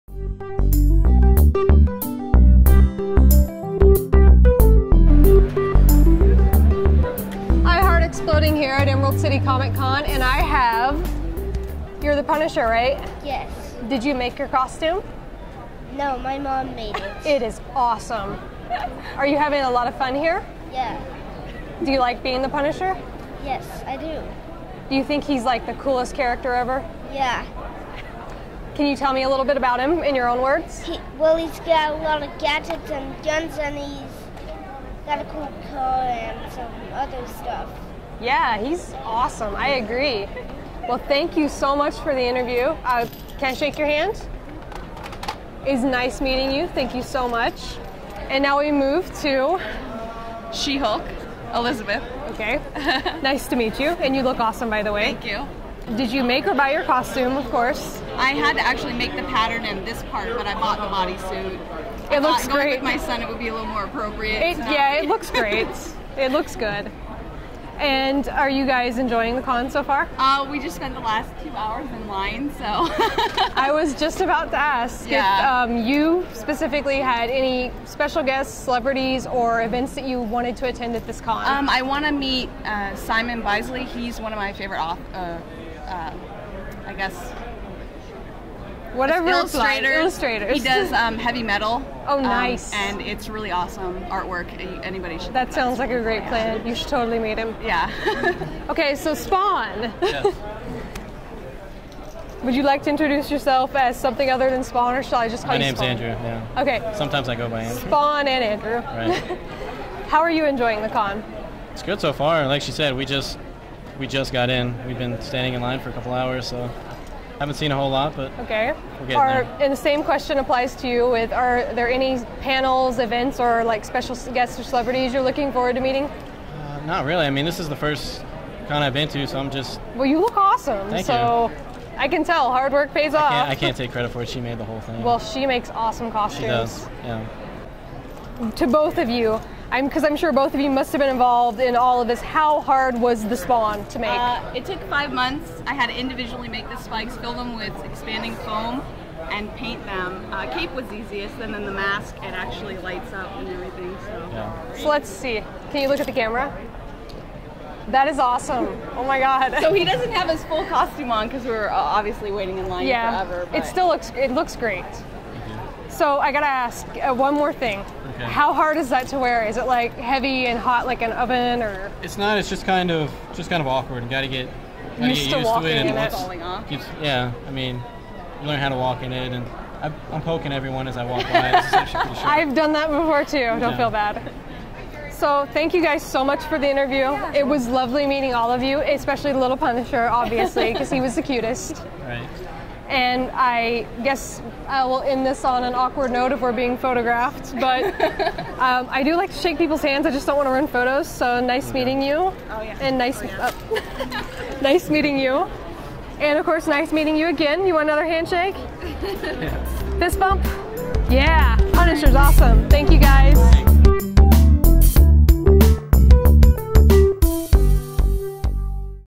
Punisher, She-Hulk and Spawn - Inside the Con - 2013 Emerald City Comicon
IHeartExploding talks to The Punisher, She-Hulk and Spawn at the 2013 Emerald City Comicon!